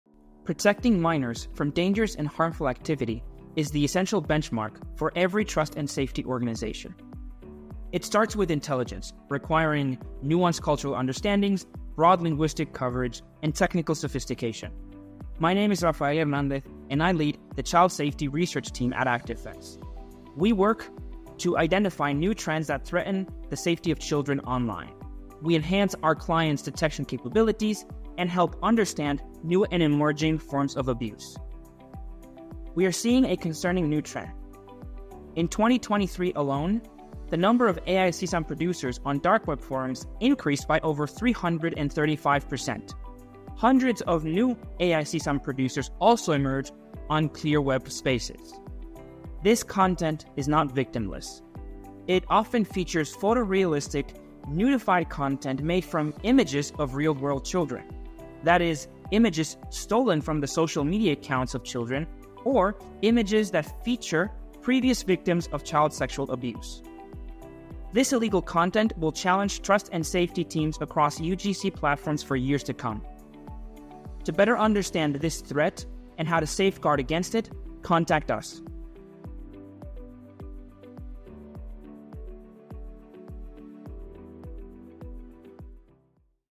AI-Generated Child Abuse Material: Trends and Safety Measures
Protecting minors from harm is crucial for any Trust & Safety team. Rafael Javier Hernández Sánchez, who leads the Child Safety research team at ActiveFence, describes his team's efforts to identify new trends threatening children's safety.